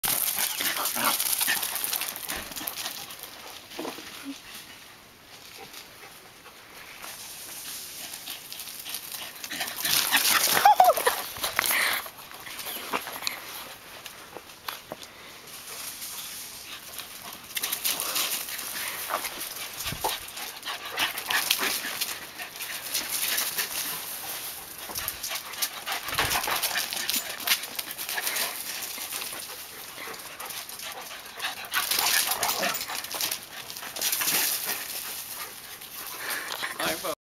Reggae Having Fun
Reggae having a play date at Joey's house.